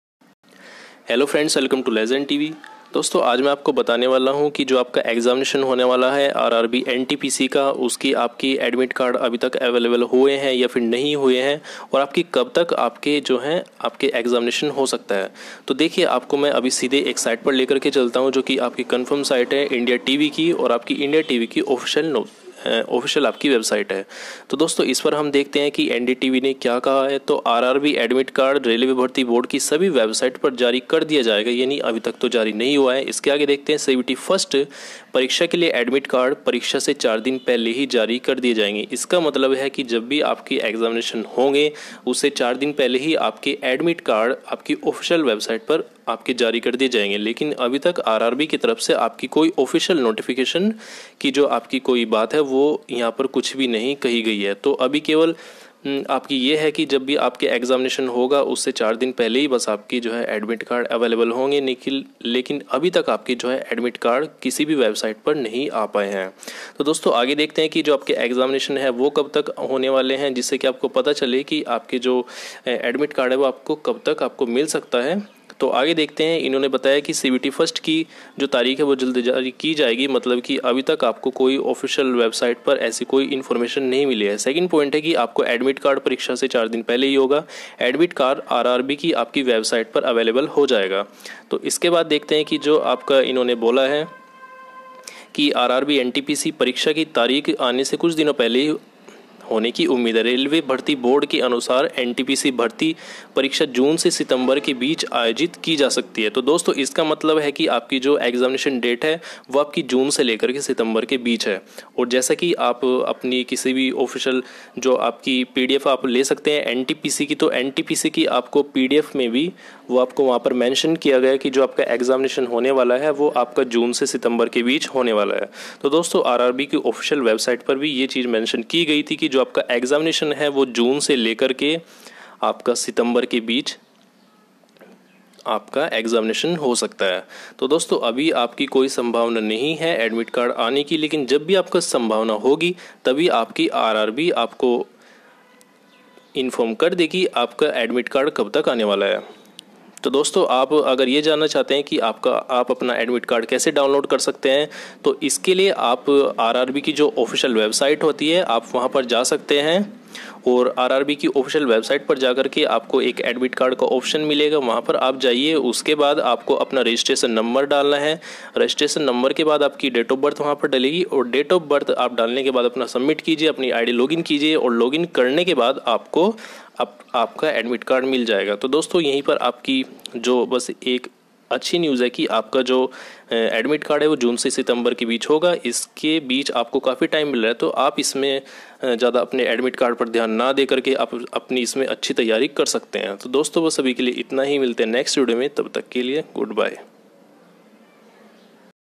RRB NTPC admit card 2019 | NTPC exam date | Ntpc admit card latest update
Here is information of RRB NTPC 2019  exam date and admit cards according to net website.
Your RRB NTPC exam date will be released soon.